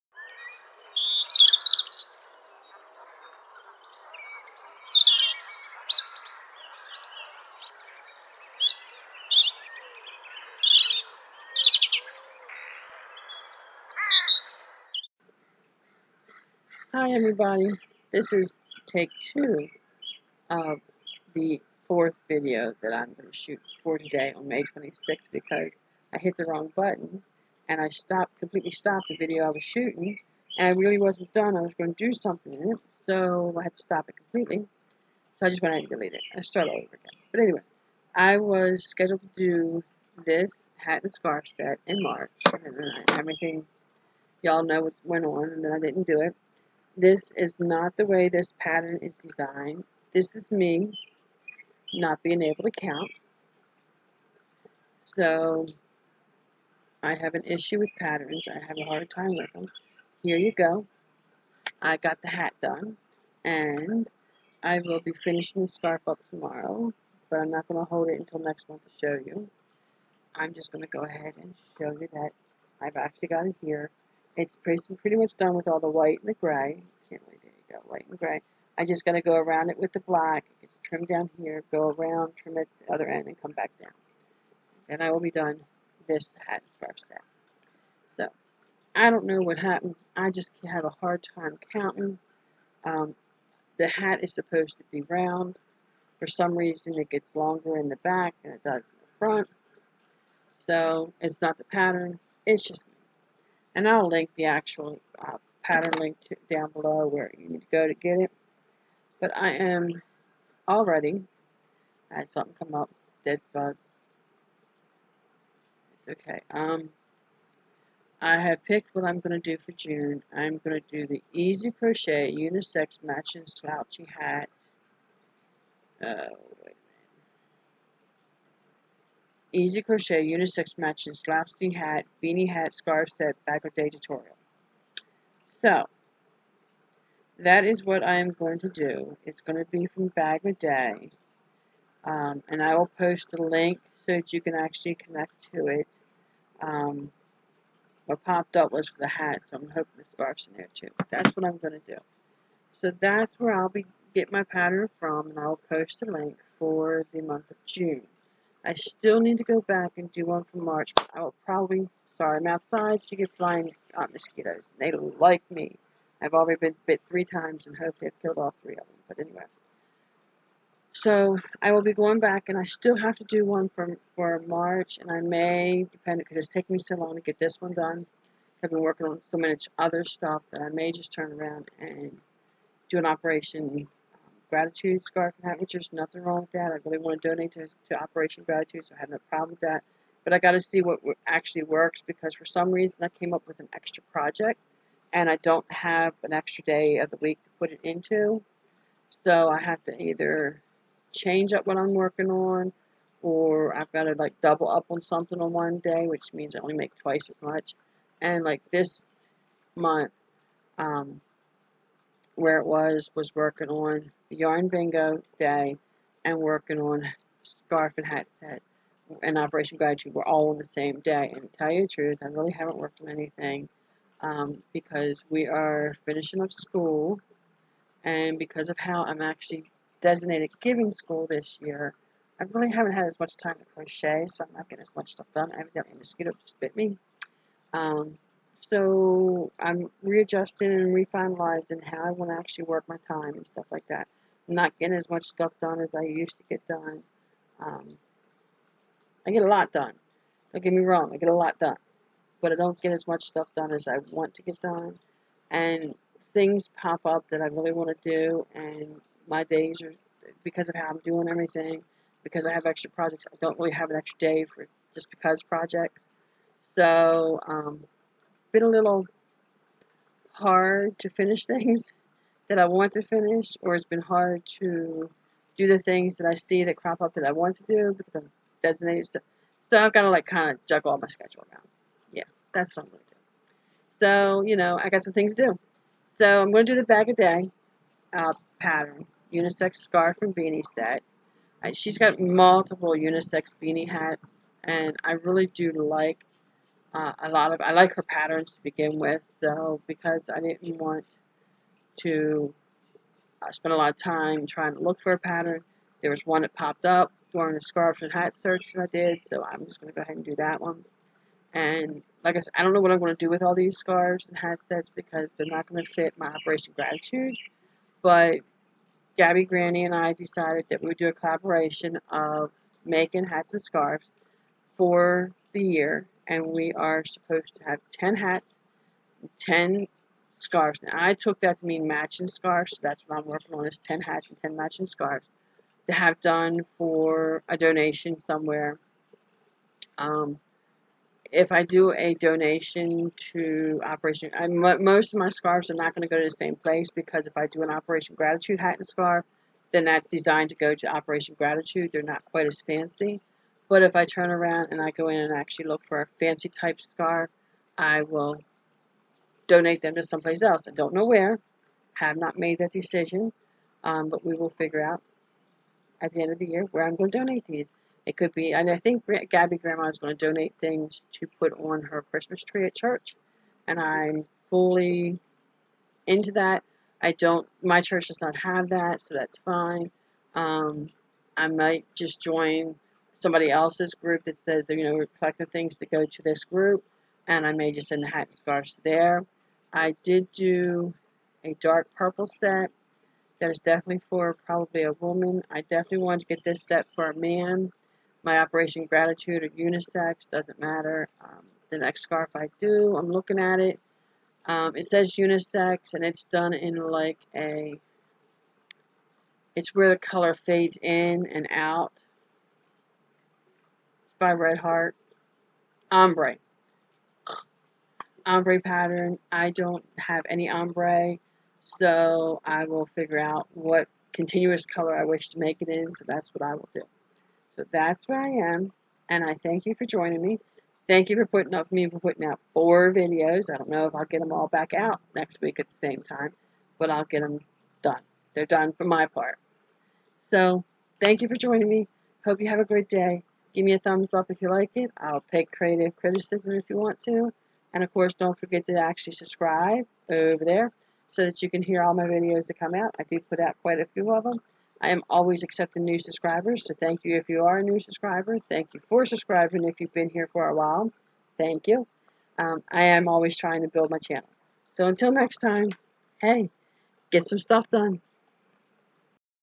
May's hat and scarf set CAL
Here is the update on the May Hat and Scarf CAL that I am doing with Gabbing Granny.  Hope you like it; the pattern is from Moogly and is called amour hat and infinity scarf and it was a crochet CAL when it published.

June's hat and scarf will be coming soon. Sorry it's so dark, it recorded lighter. Lol

Robinflying Crochet Kirkpatrick